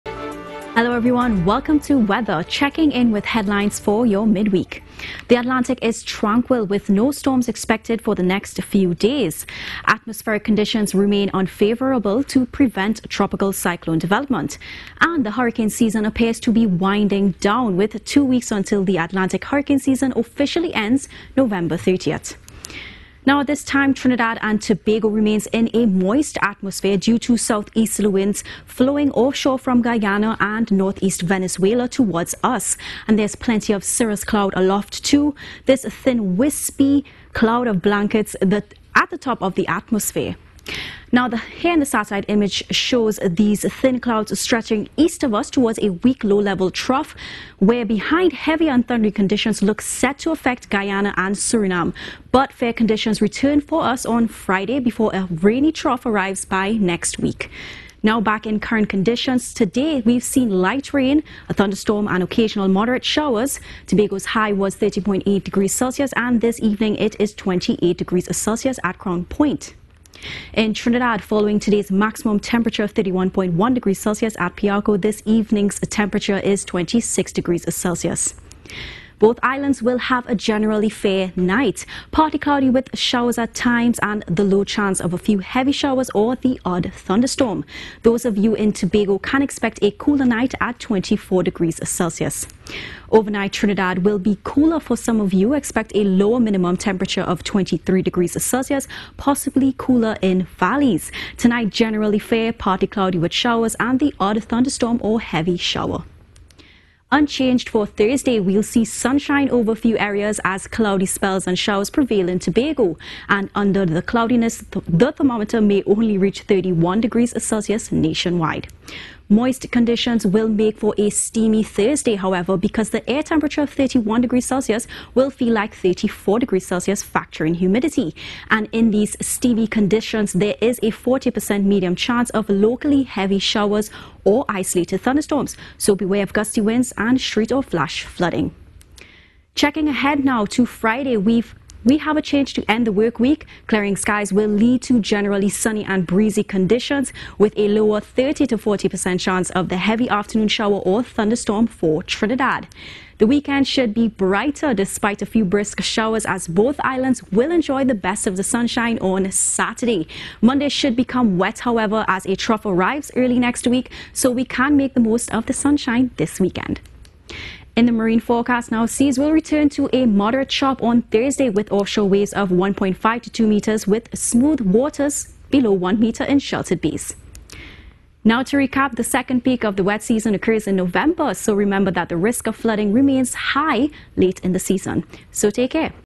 Weather Outlook - Wednesday November 16th 2022
The Hurricane Season appears to be winding down, with two weeks until the Atlantic Hurricane Season officially ends November 30th.